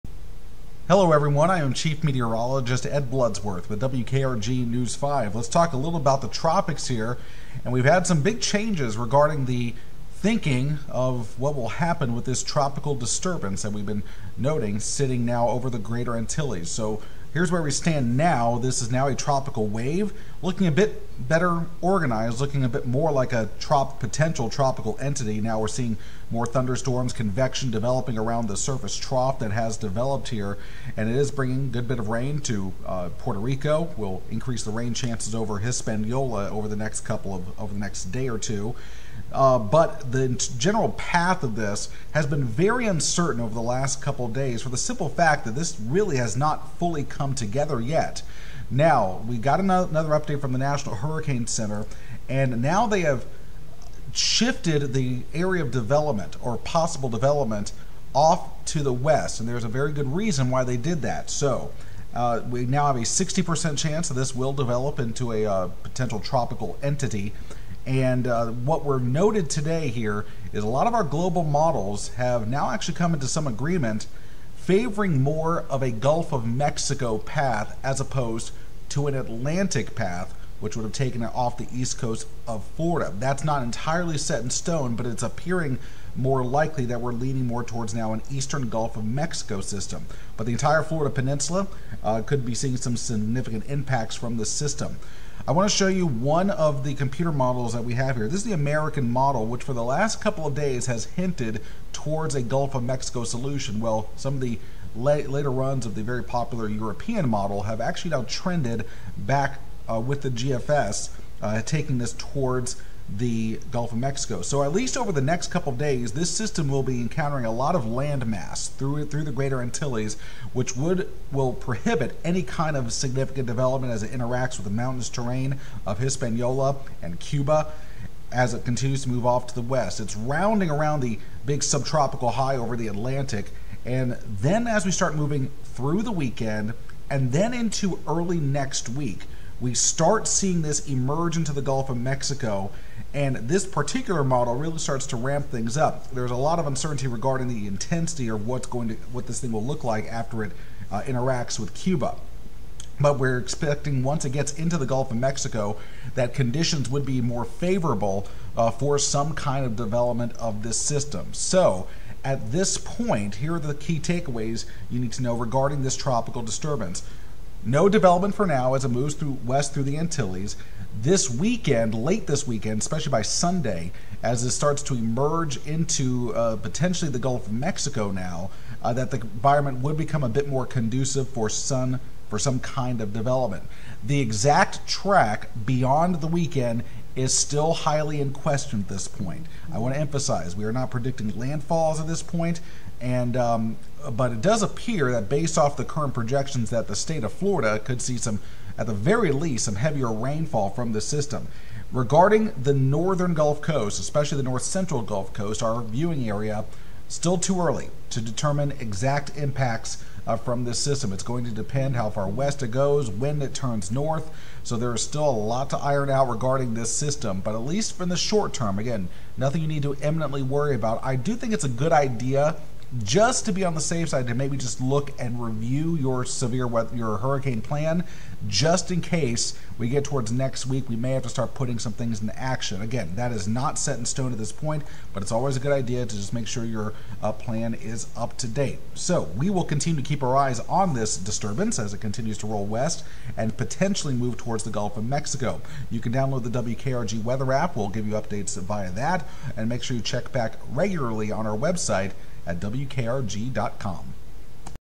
Tracking Tropical Disturbance, Could Enter the Gulf of Mexico
A tropical wave is slowly becoming better organized as it approaches the Greater Antilles of the Caribbean. Conditions are expected to become more favorable for development as it approaches the Turks & Caicos and the Bahamas. A tropical depression could form by the weekend. The eventual track of the system will depend on when it finally comes together. If it forms late and remains weak, the system could take a more westerly track possibly into the eastern Gulf of Mexico. The majority of models favor a track up the east coast of Florida.